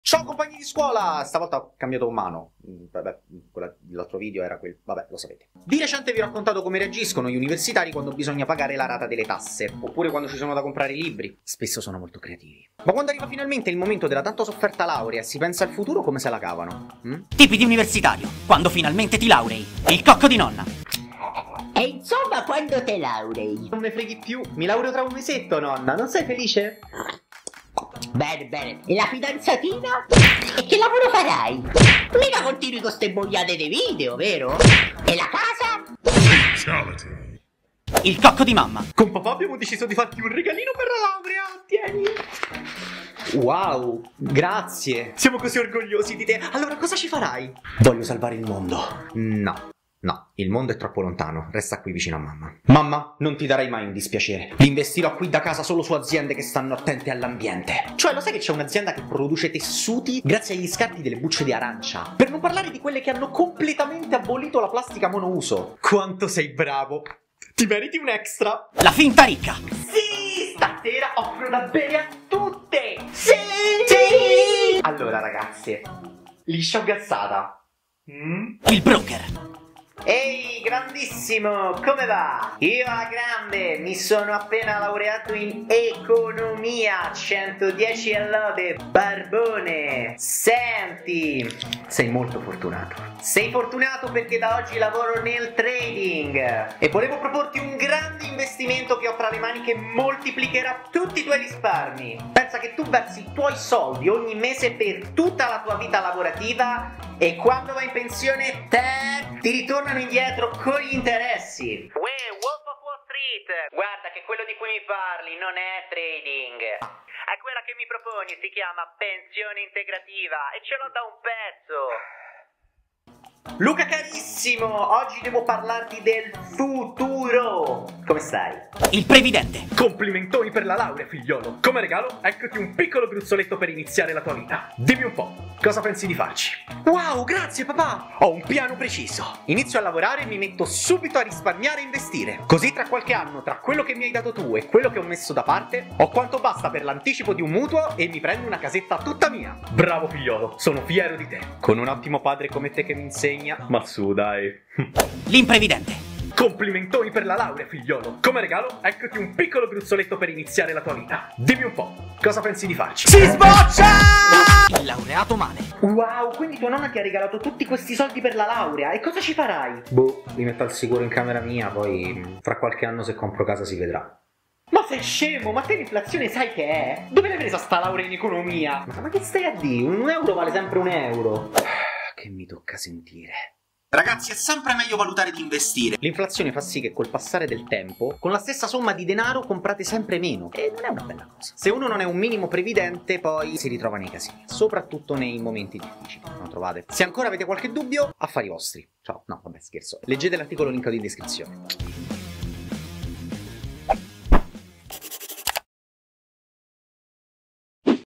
Tipi di Universitario | Quando finalmente ti laurei - Nikolais ft. Unicredit
Quando finalmente arriva il momento della laurea è fantastico! Ma è davvero tutto rose e fiori...?